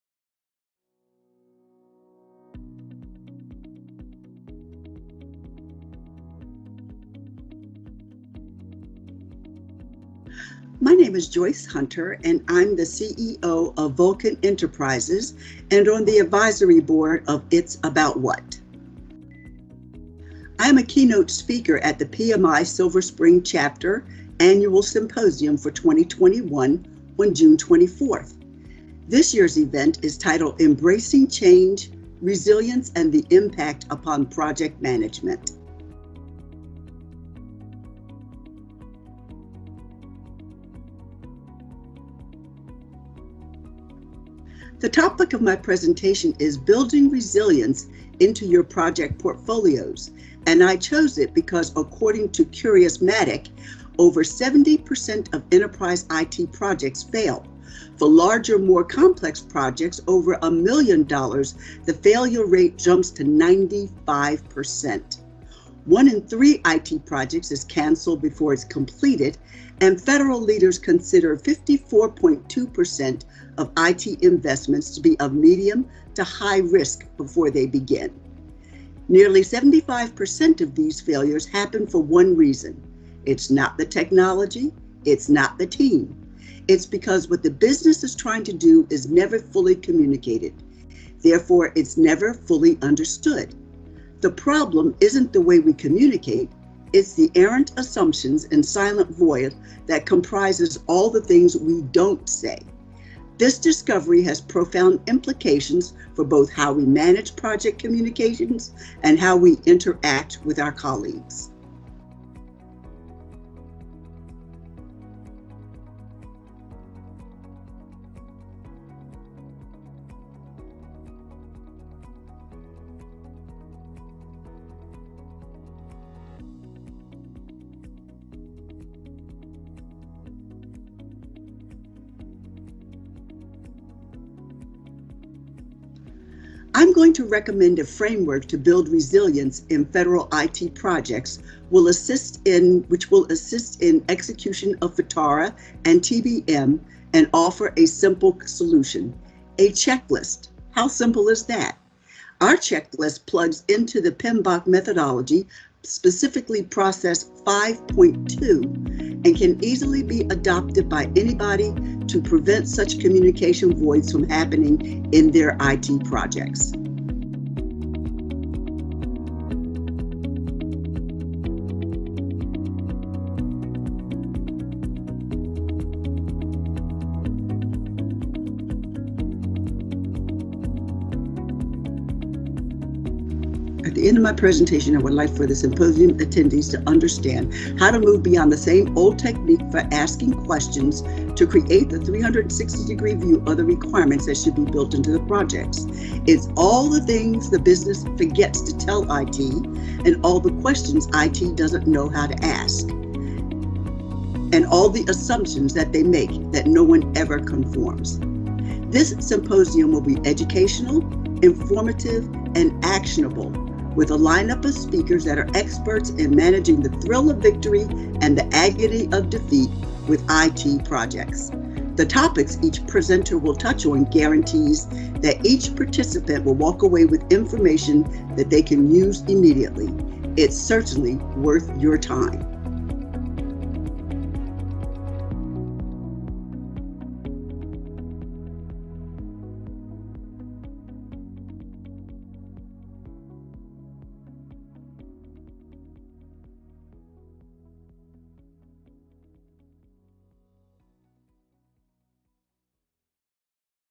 2021 PMISSC Symposium Promotional Video with Joyce Hunter
As the Project Management Institute Silver Spring Chapter (PMISSC) prepares for its 8th annual symposium titled, "Embracing Change: Resilience and the Impact Upon Project Management" on June 24, 2021, Joyce Hunter, one of the two keynote speakers, tells registrants what he'll talk about and what the entire event holds for them all!